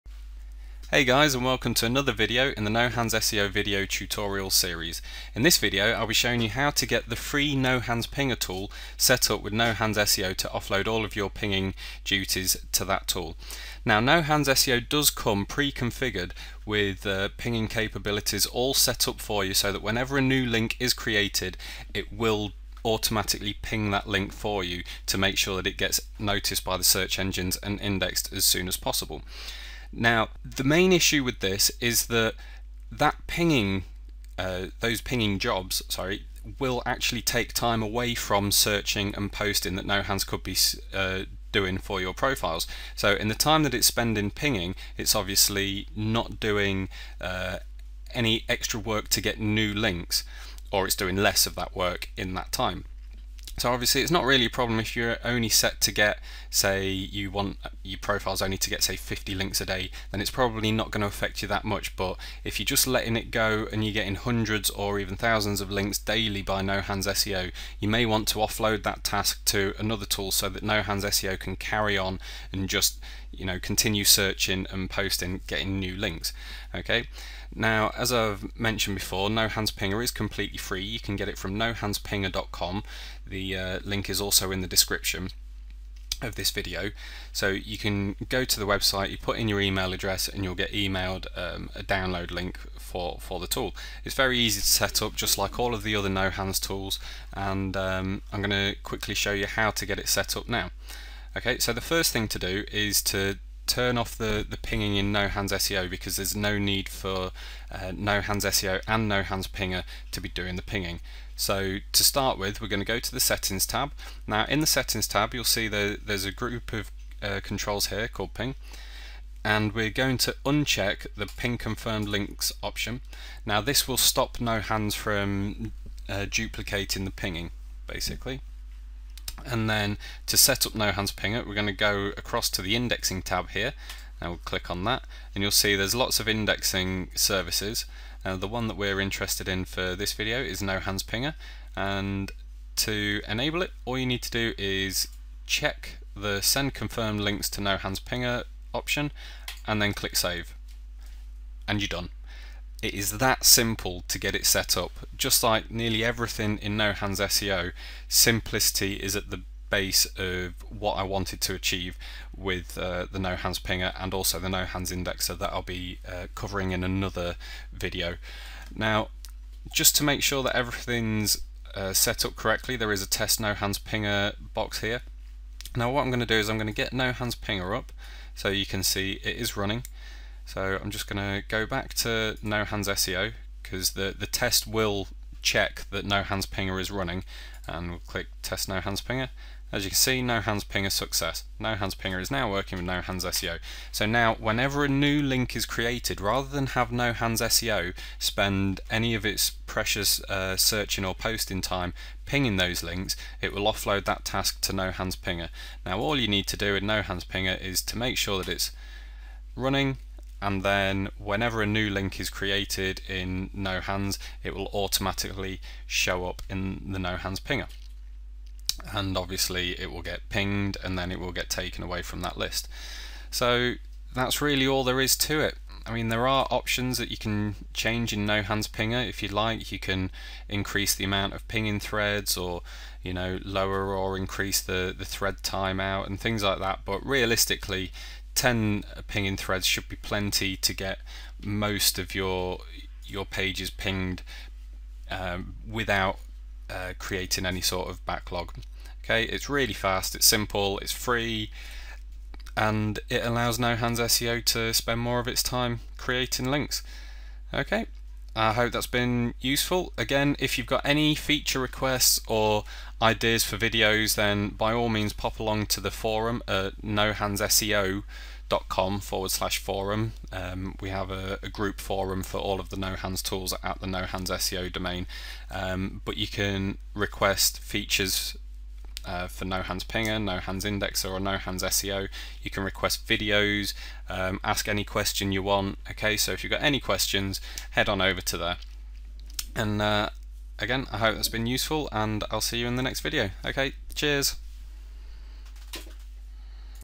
No Hands Pinger Integration with No Hands SEO
This video shows how easy it is to offload all of the pinging tasks for your newly created links in No Hands SEO to the free No Hands Pinger tool. This will allow No Hands SEO to spend much more time creating new links and will not cost you an extra penny.